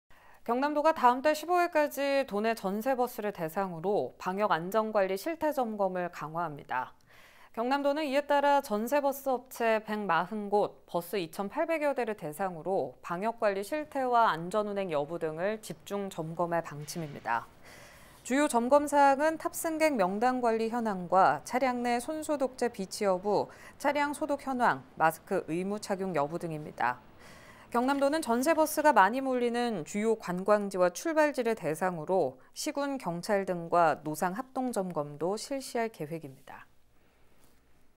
경남도, 전세버스 '단풍 방역' 강화
경남도가 다음 달 15일까지
도내 전세버스를 대상으로
방역·안전 관리 실태를 점검을 강화합니다.